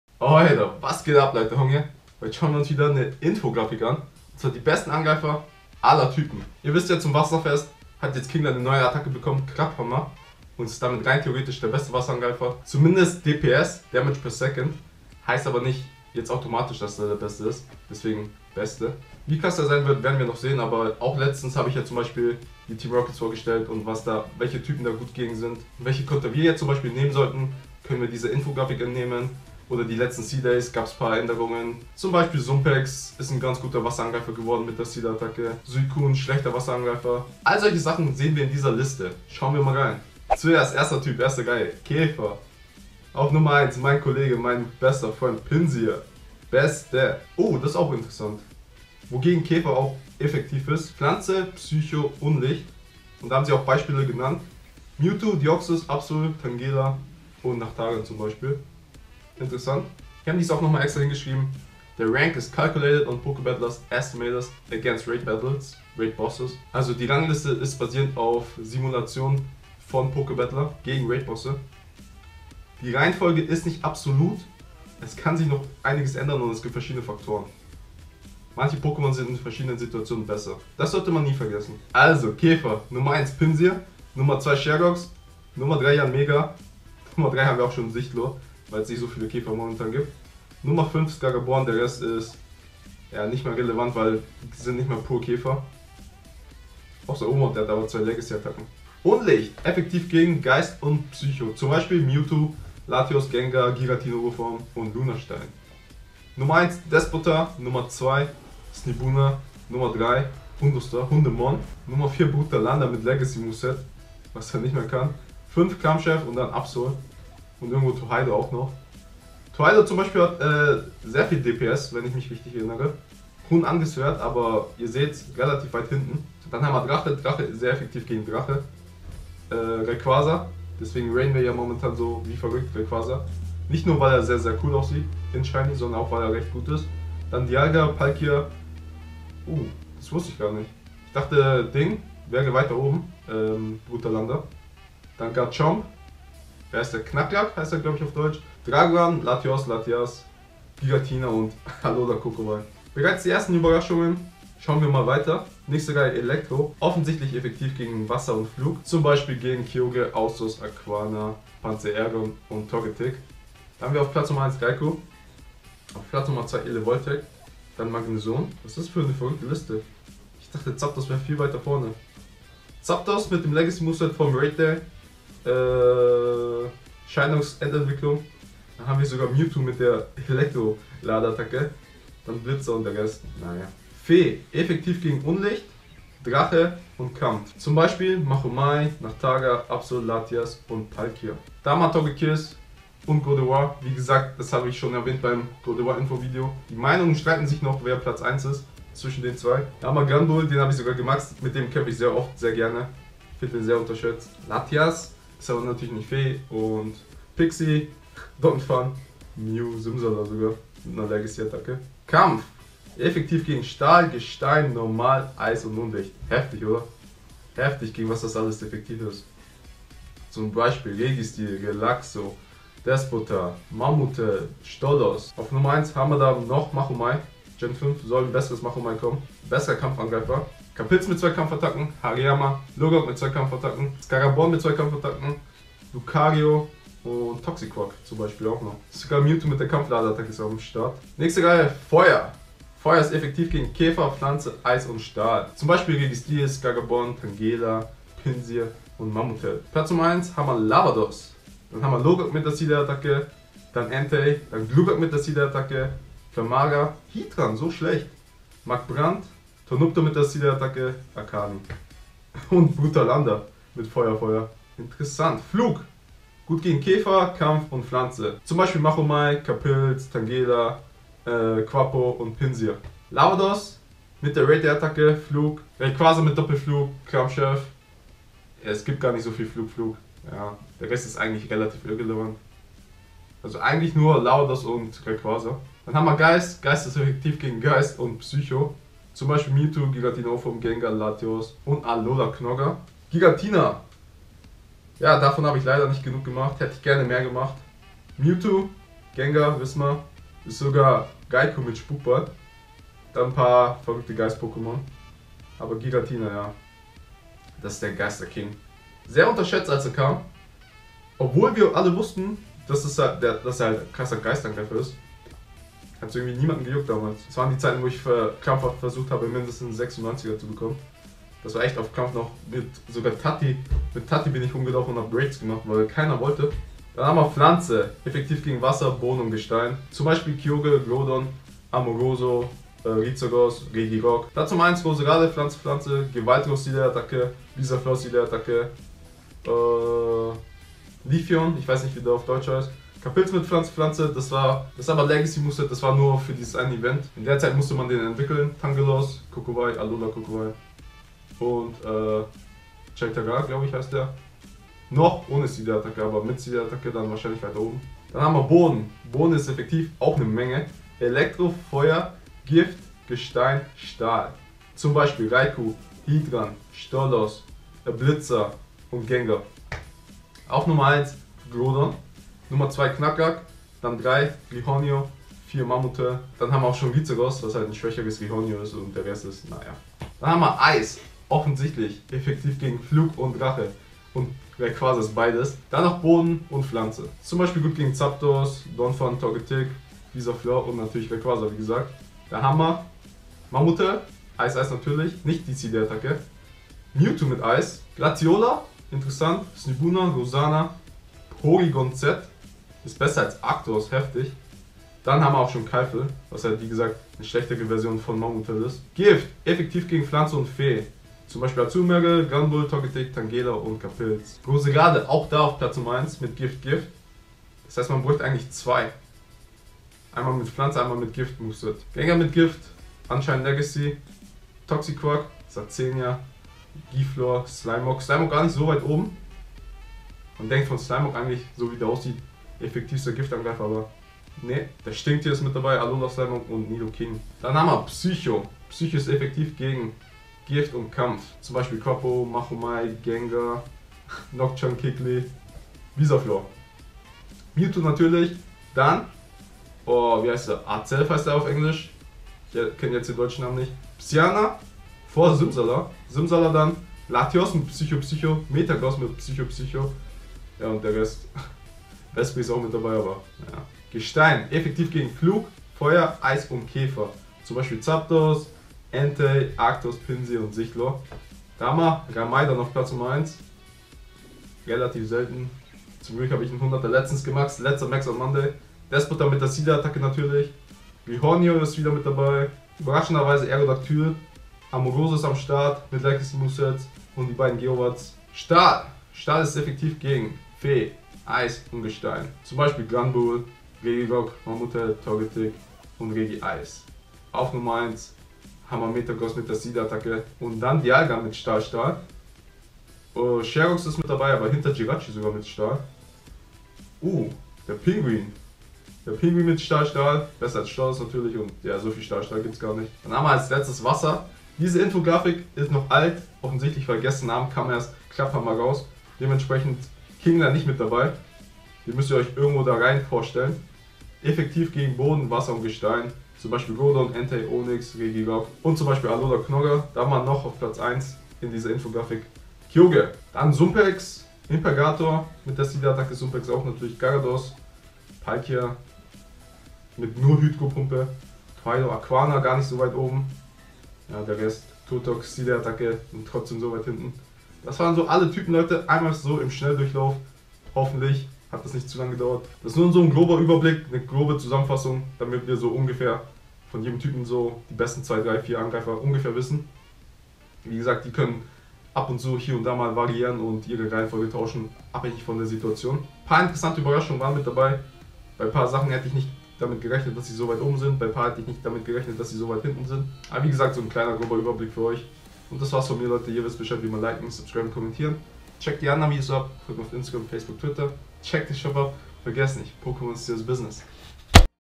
Top 10 ANGREIFER jedes Typen in Pokemon Go!
euda hier mal die Top 10 Angreifer jedes Typen für euch
ein kleiner Überblick um mal zu sehen was gute Konter sind
hier findet ihr die Infografiken: